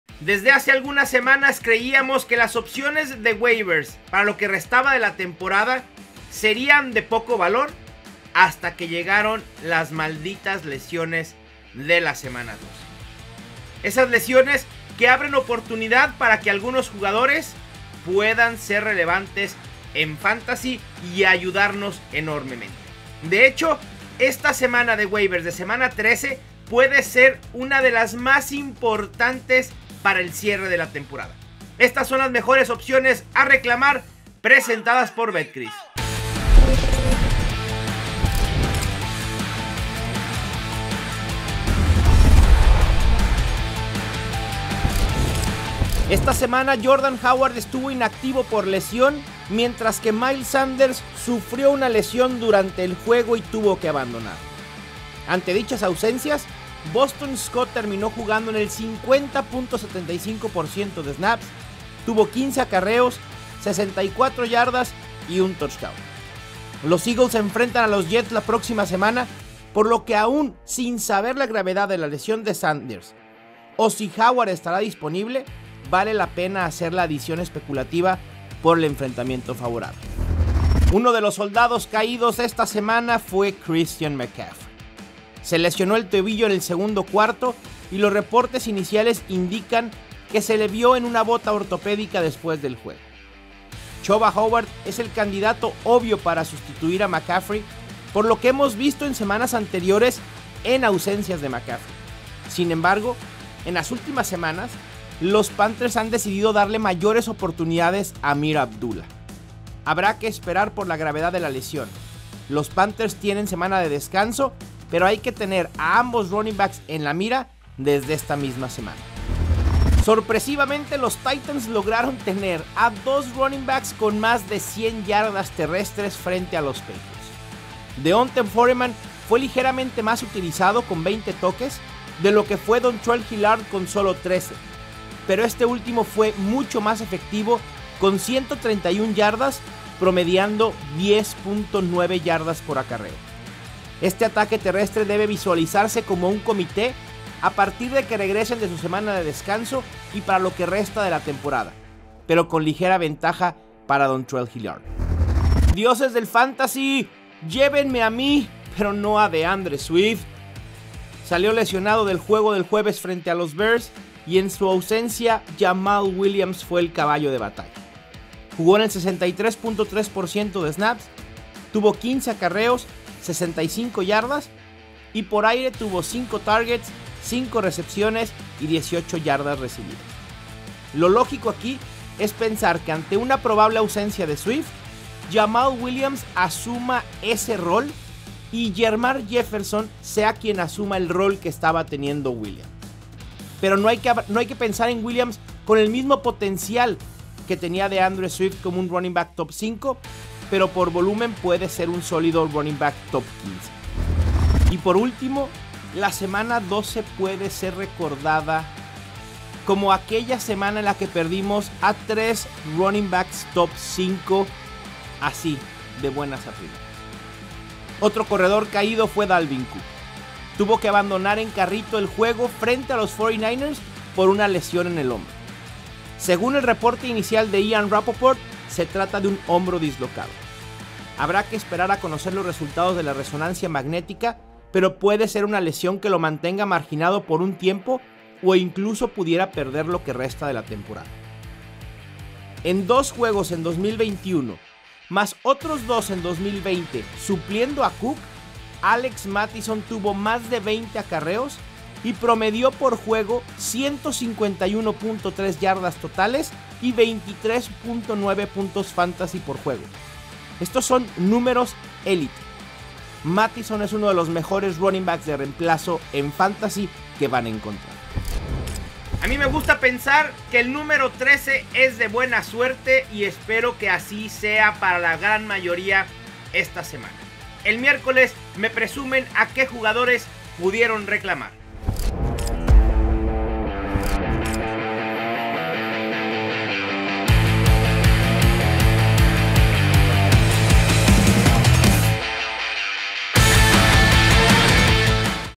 Jugadores que DEBES tomar en WAIVERS | Semana 13 | NFL Fantasy en Español
La Semana 13 está aquí y llegó el momento de que Mauricio Gutiérrez te comparta las mejores recomendaciones para los waivers. No importa si comenzaste tu temporada con triunfo o derrota, en los waivers es donde se gana el NFLFantasy